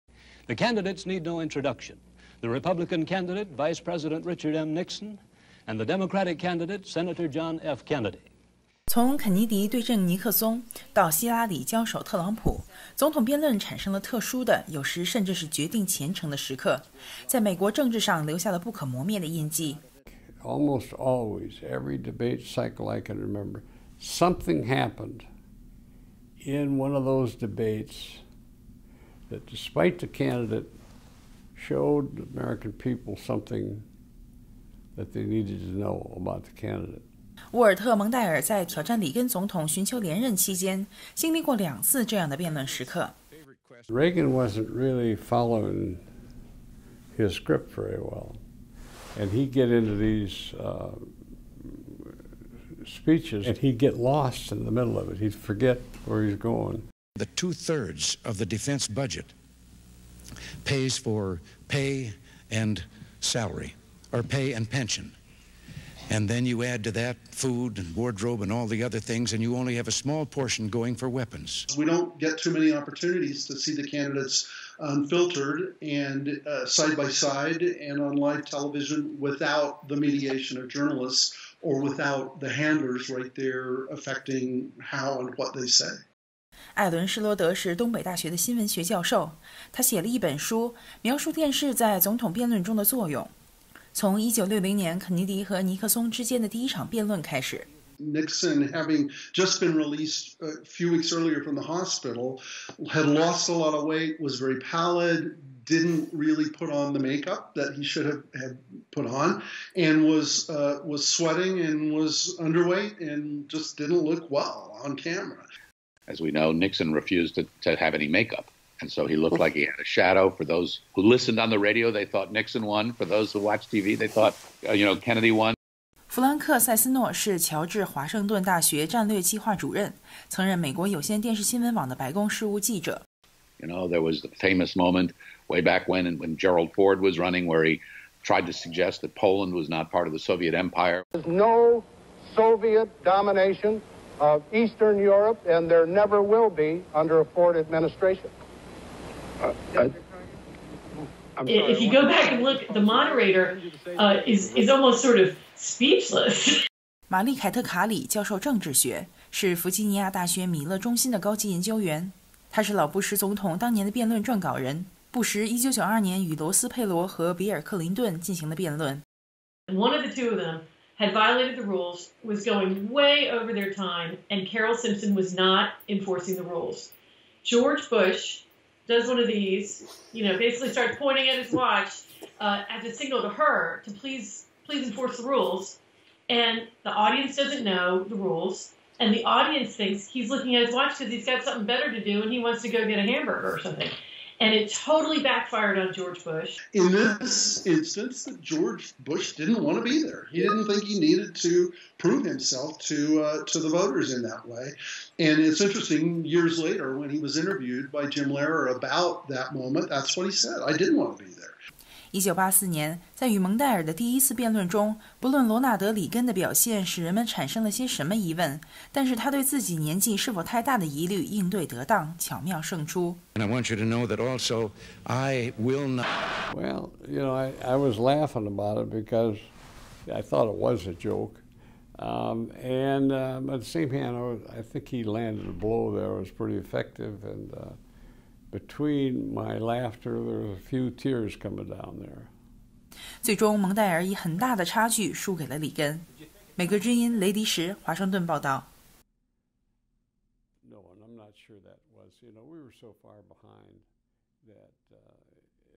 美国总统辩论回顾
避免出现重大错误是特朗普总统和前副总统拜登在9月29日星期二进行首次辩论中的主要目标。历届总统辩论都有塑造美国政治的历史性时刻。一起跟随美国之音记者雷迪什来重温那些时刻。